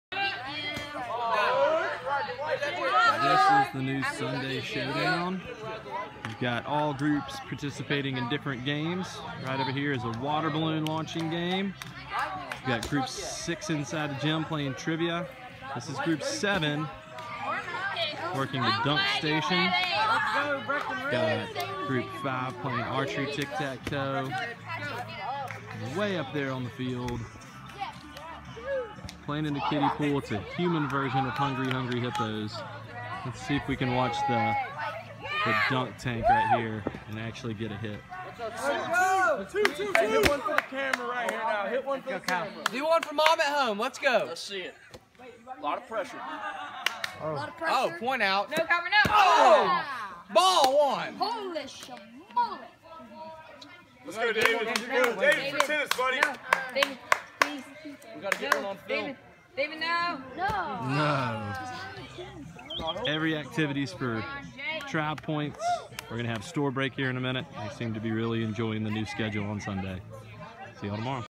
Daily Video - 6/12/2016
We changed our Sunday afternoon schedule again this summer.  The campers still compete for Tribe Points; however, this year the games were more of a carnival style than traditional Laney games.  The boys competed in a dunk tank activity, water balloon launching, trivia, archery tic-tac-toe, giant Twister and more!  You can tell they had a great time and the Comanches had a big day winning 475 points!